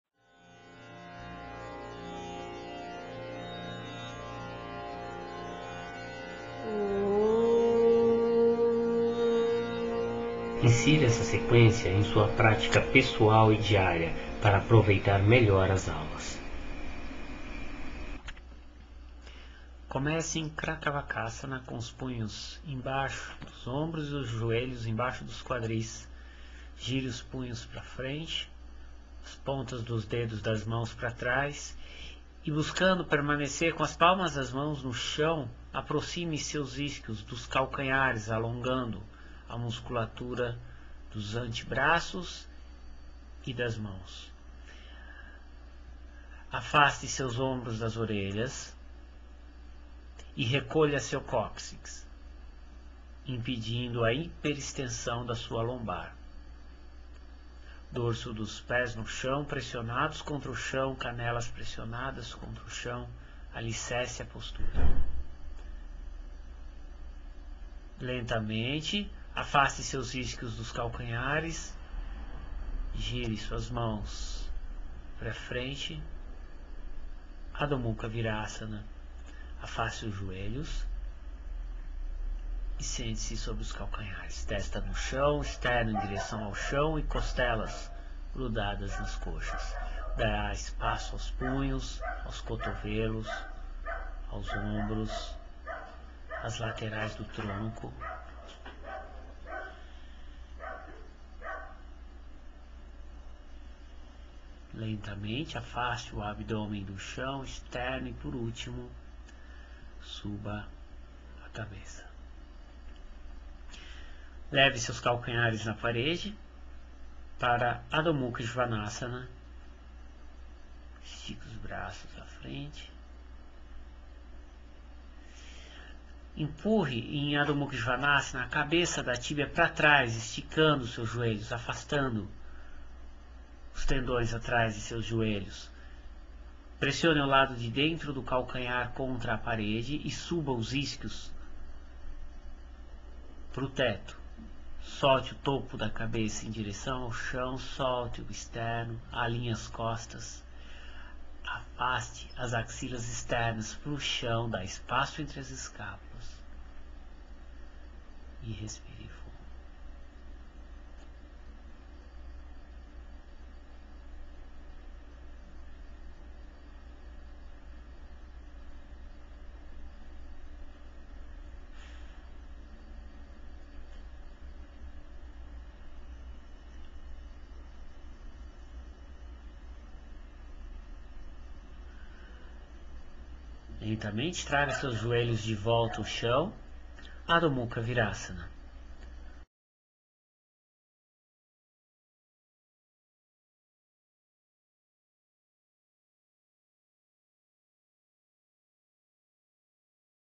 Prática Pessoal de Yoga 20/04/09 - Parte I
Prática pessoal de Yoga referente à terceira e quarta semanas de abril. Postura em destaque: Mayurasana. Primeira parte do total de três.